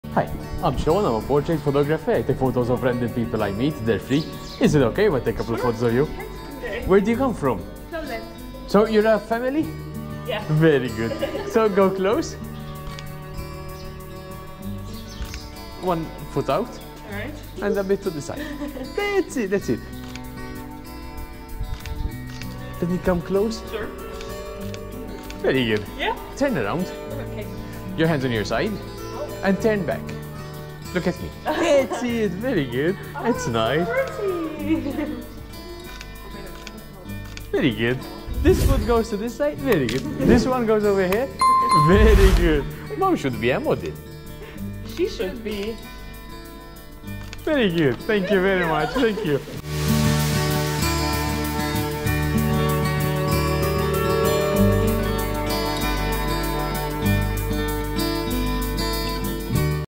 Street photography happy family Poland
Wow!! I met these gorgeous, pretty girls from Poland while shooting some street photography in Mdina, Malta. I asked for some photos and they immediately agreed. they where very happy and had magnificent smiles. I started with a group photo and after shot some portraits each. I gave them some posing tips to make them look good although they where very photogenic. They really like posing so I gave them some posing tips during the photoshoot.
The photos turned out great and short video of the street photography in POV style. I also gave several street photography tips during the shoot.

GoPro Hero 11
Latest GoPro Hero 12
Sony A7Riv
Sony 85 F1.8
Sony 85mm F1.4
GoPro chest strap
GoPro 3-Way Grip